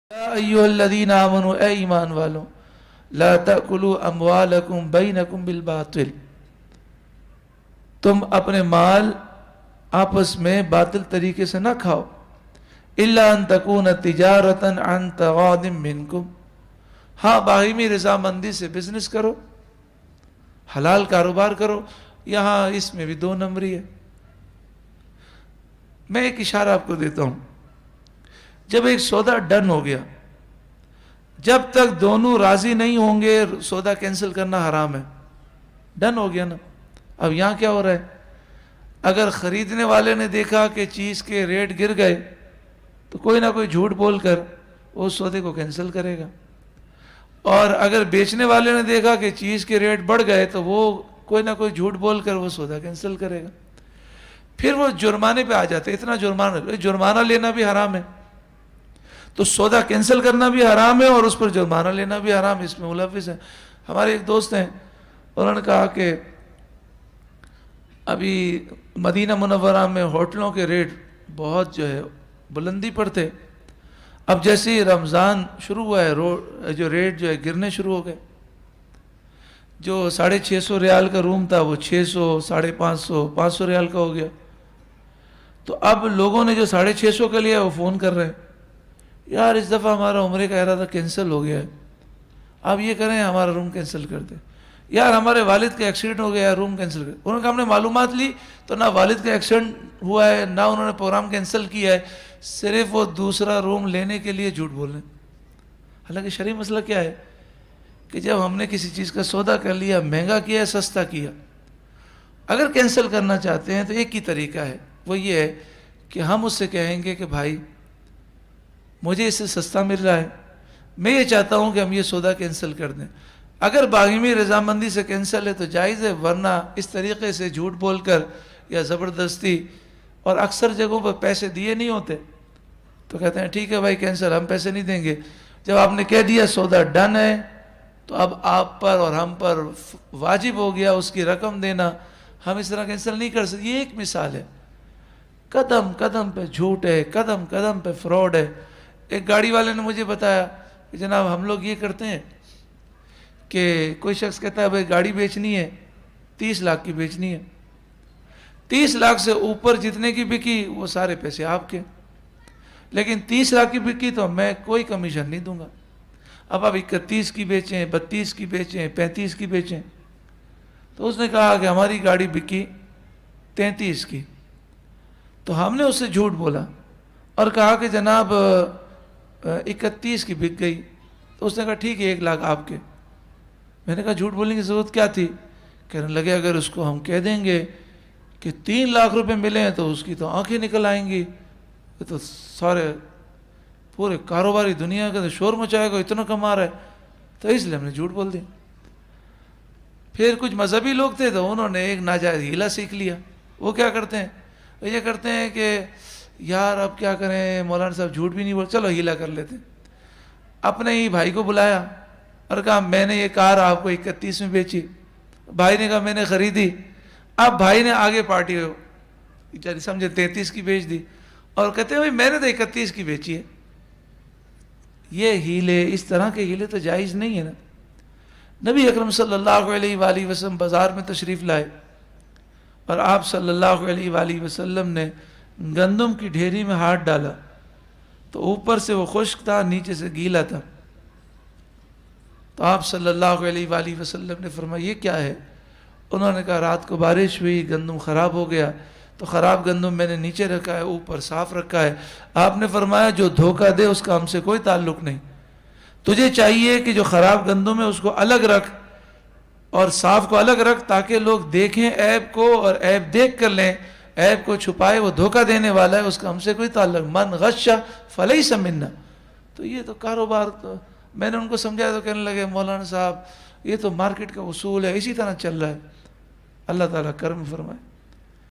Karobar me Dhoka Aur Deen_E_Islam || Short Clip || Mufti Asif Abdullah Qadri
Ｙａａｄｇａａｒ Ｒａｍｚａｎ （Ｓｈｏｒｔ Ｃｌｉｐ）

Thanks Every one for your LOVE ❤ and Support 👍